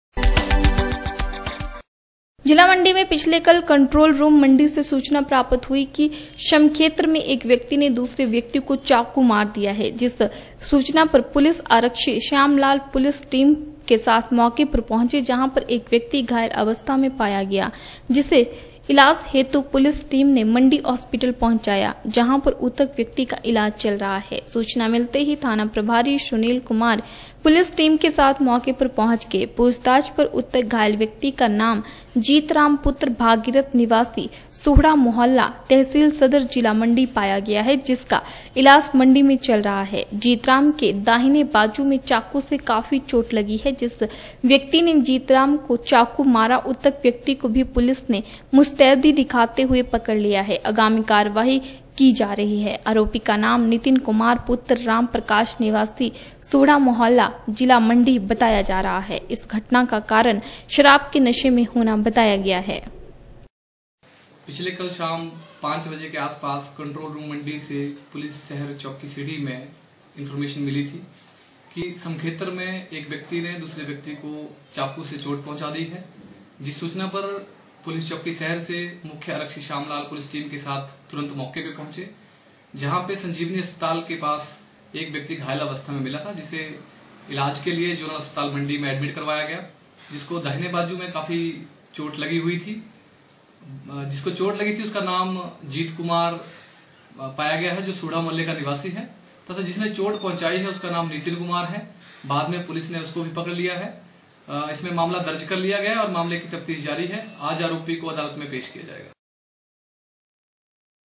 शाम को कंट्रोल रूम मंडी से सूचना प्राप्त हुई की समखेत्र में एक व्यक्ति ने दूसरे व्यक्ति को चाकू मार
पिछले कल शाम को कंट्रोल रूम मंडी से सूचना प्राप्त हुई की समखेत्र में एक व्यक्ति ने दूसरे व्यक्ति को चाकू मार दिया है जिस सूचना पर मुख्य आरक्षी श्यामलाल पुलिस टीम के मौके पर पहुंचे जहां पर एक व्यक्ति घायल अवस्था में पाया गया जिसे इलाज हेतु पुलिस टीम ने मंडी हॉस्पिटल  पंहुचाया जहां उक्त व्यक्ति का इलाज चल रहा है ।